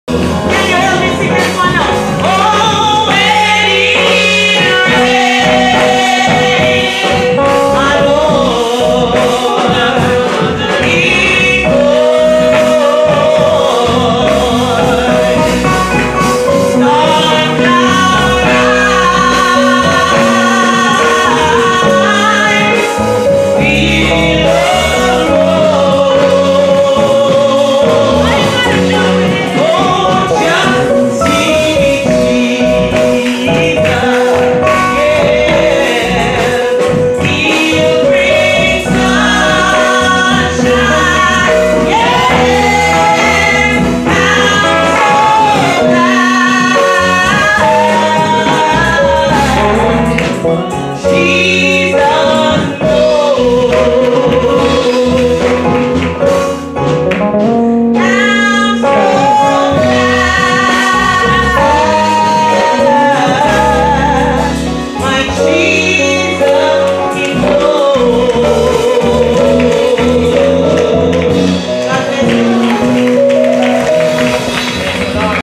Danesha singing at Heathfield Seventh Day Church of God . . . Angelic Voice
Singing when it rains . .